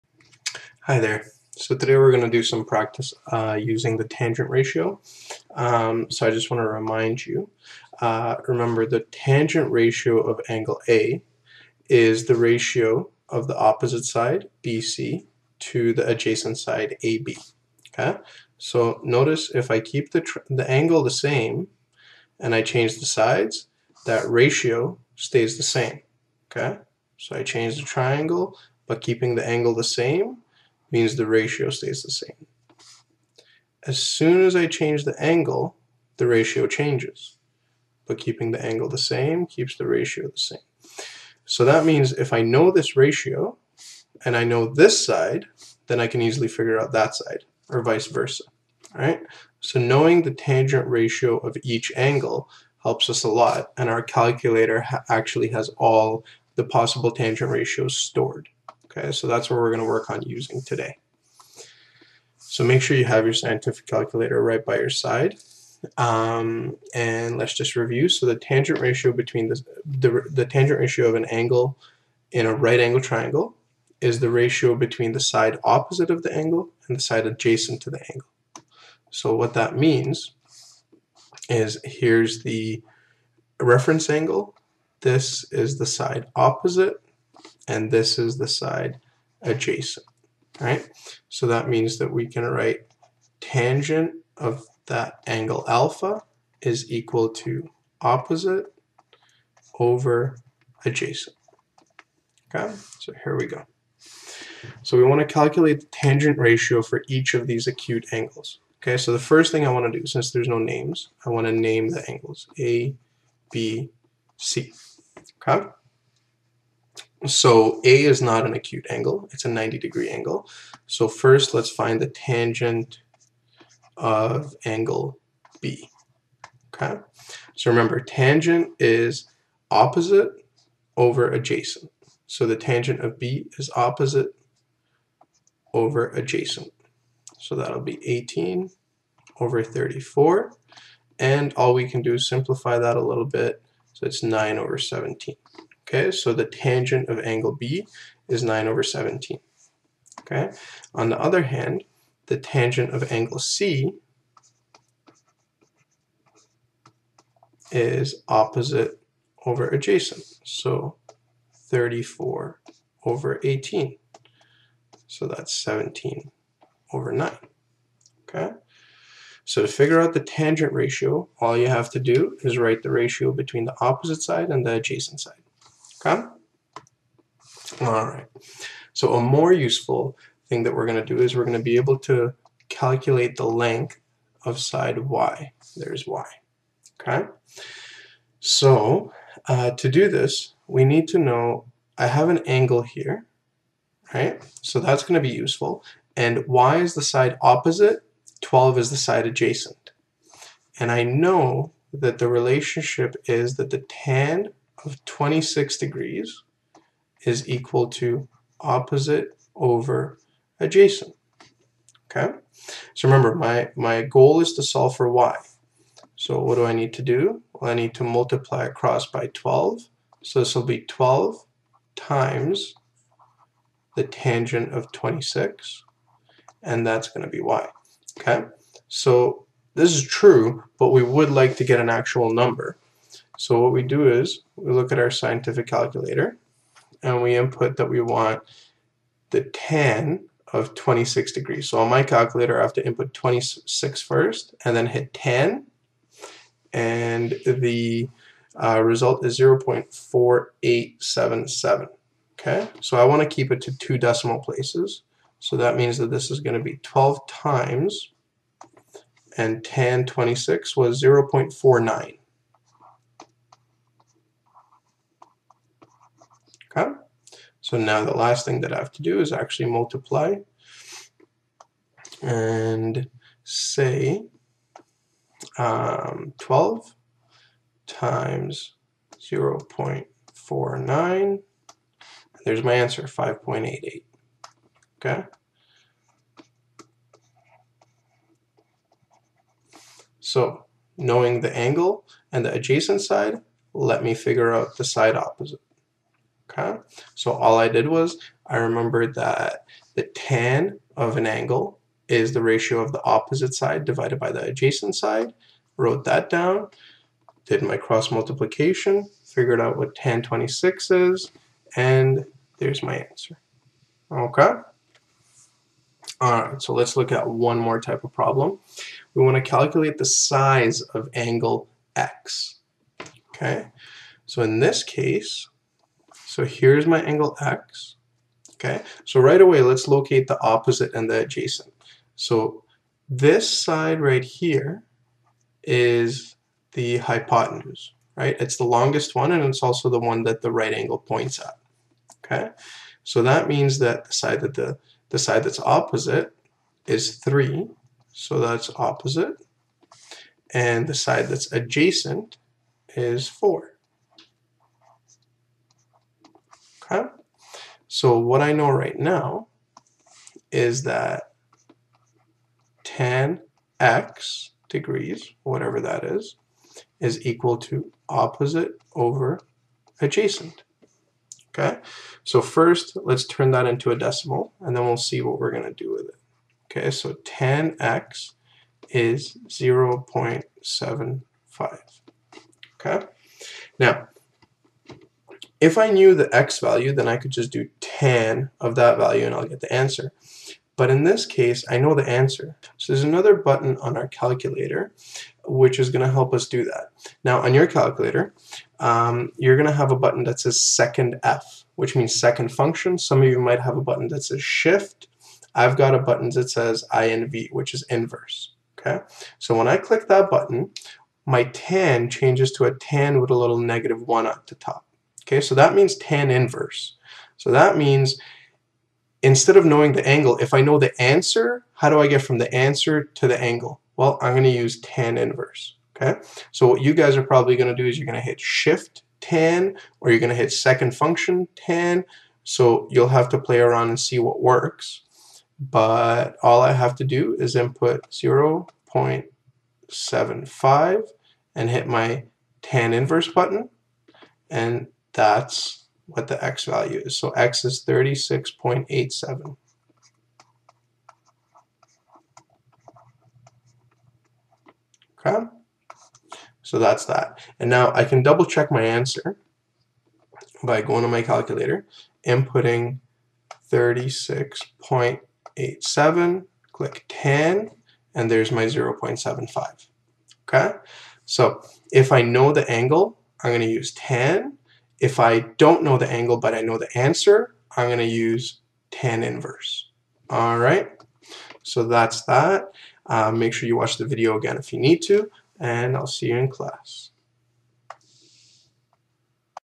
2.4 Tan Ratio
A demonstration of why the Tan ratio is important and also some examples regarding how to use the ratio to find the lengths of sides and the sizes of angles.